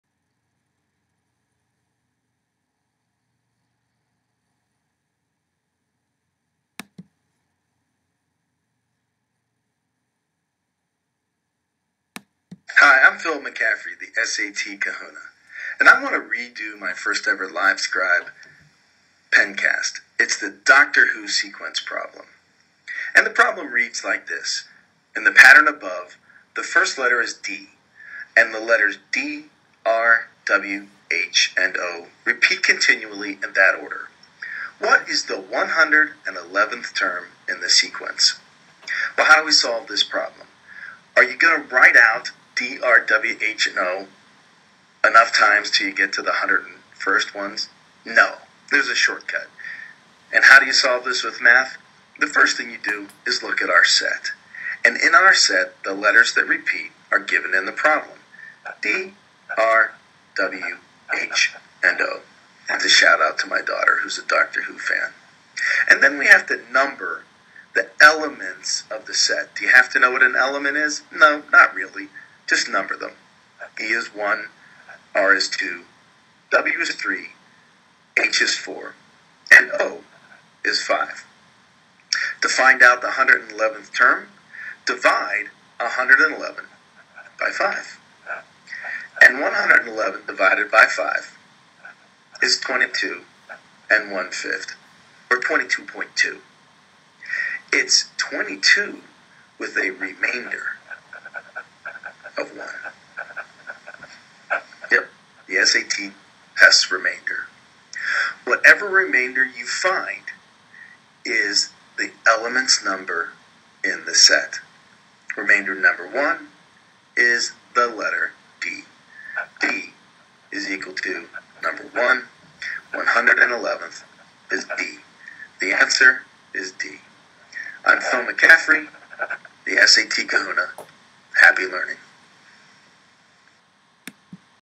SAT/ACT/GRE Math Repeating Sequence
This is a common problem on the SAT/ACT/GRE and all other standardized tests.  To solve:   1. Count the number of things (elements) that they tell you are repeating.                                    2. Divide the term they want you to find (here 111th) by the number of things (elements) in the set.                                                                3. If there is a remainder (usually is) count out the elements by the remainder and that is the answer.                                                                   Happy Learning